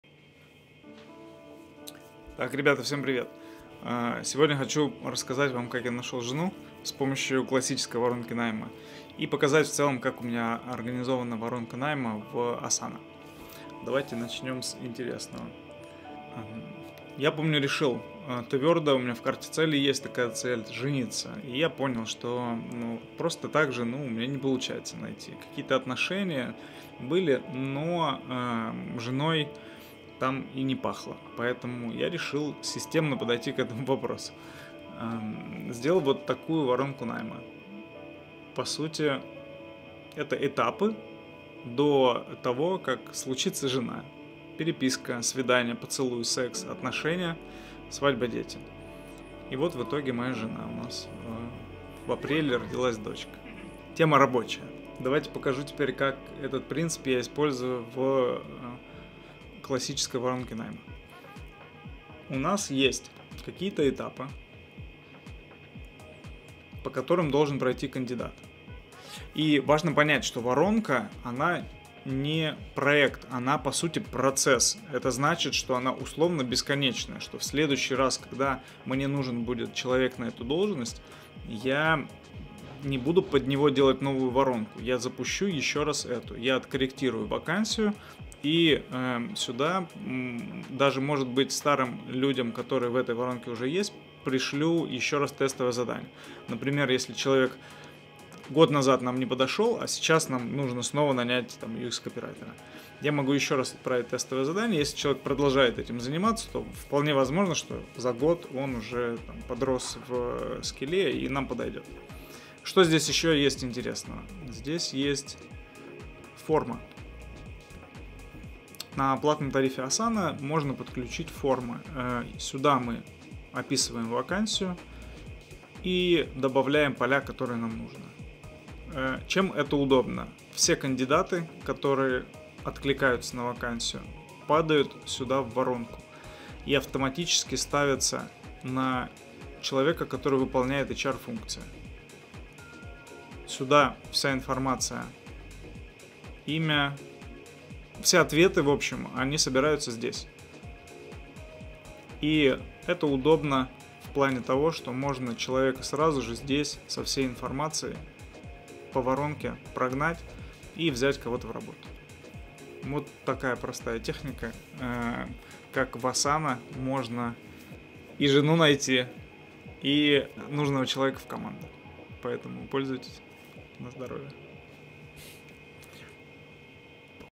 Как найти жену или сотрудника через воронку найма в Asana
Короткое видео, как я нашел жену и как организовал воронку найма в Asana.